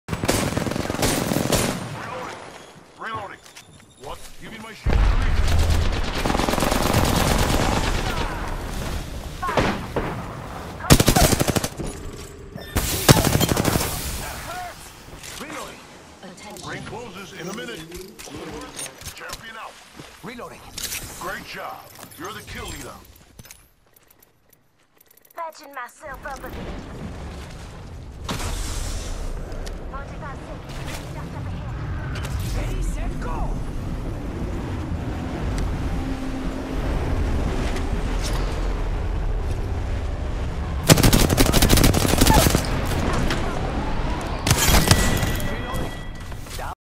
Squad Wipe But Watch The Last Guy. Dies While Respawned EPIC MOMENT
Wipe a Team Then Kill a Dude Whos Being Respawned From The Plane.